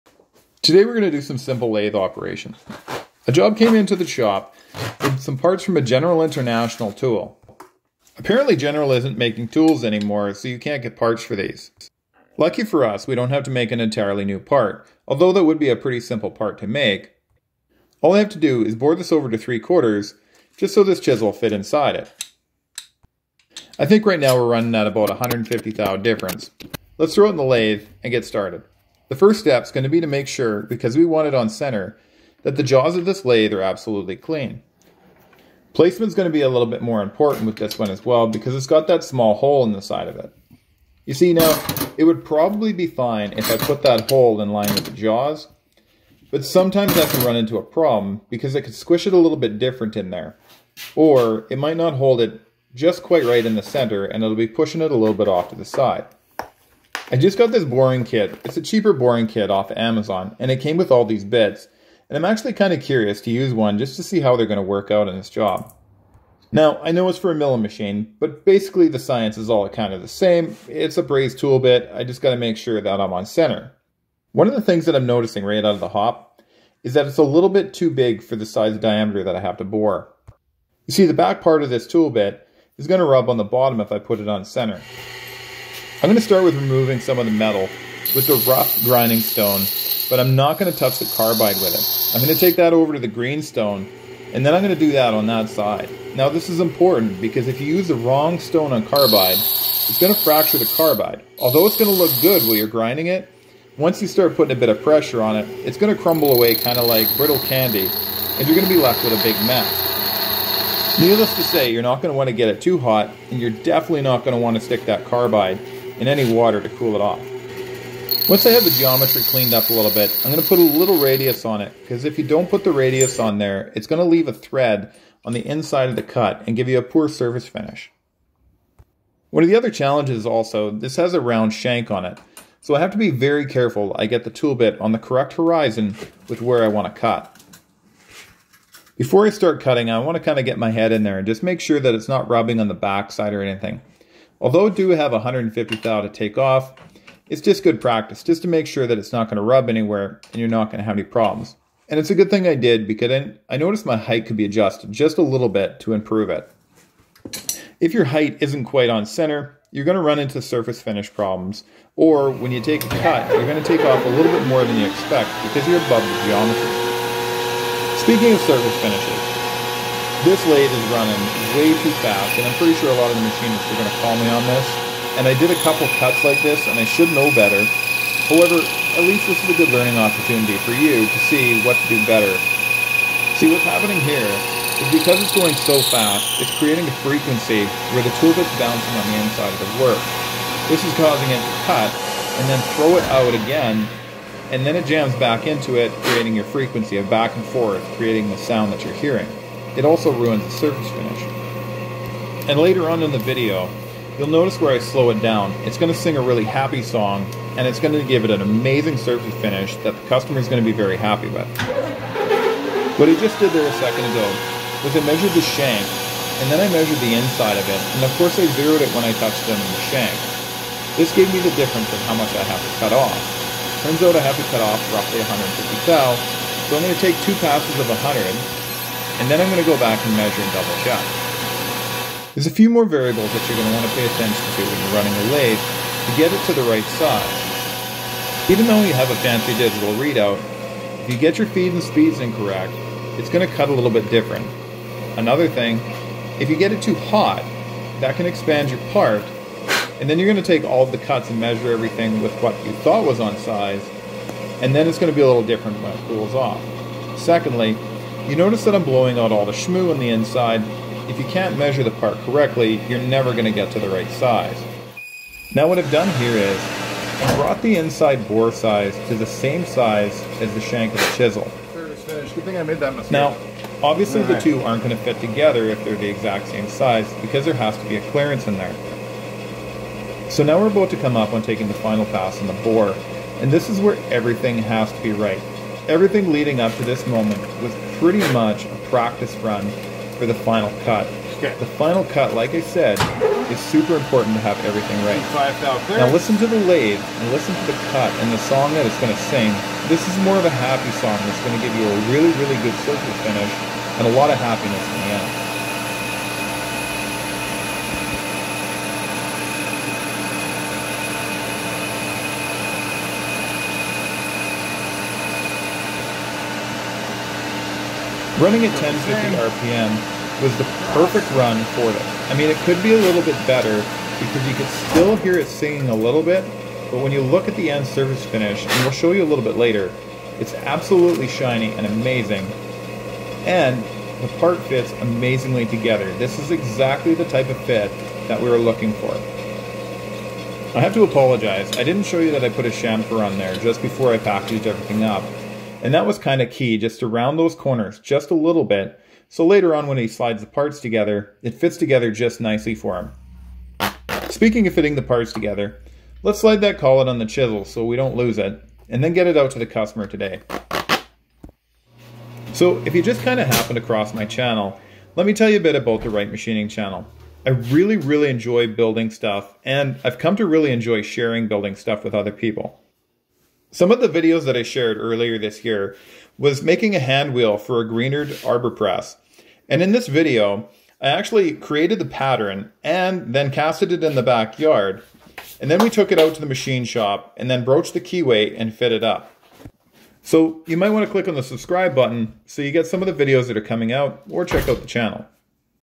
Boring Accurate Holes on Manual Lathe for a General Mortis Collet
In this video, we do a basic lathe operation to bore a hole in an existing part. We will talk about correct ways to do things and point out incorrect ways and correct ways as some mistakes are made and corrected.

The job is taking a collet and we bore lathe oversize to .750. this is to fit a mortise chisel for a General Mortis Machine. As some may know you can no longer get parts for these machines

  A customer brought in the other day a bushing/call it for a General Mortis machine. Lucky for him he had purchased a second call before the company went out of business Place another one he had worn out by using for another purpose.

Thank you to Jeffery A. ( a Tool and Die Maker ). He brought up that running a dial indicator was missed in this video. It is good practice to put a dial on a part, even if it is a self-centering chuck. Your self-centering chuck generally will get you close if everything is set up correctly. However, you can still be out if some factors are missed like for example,  burrs on the part, oval part, dirt in the chuck teeth or even the scrolls inside the chuck.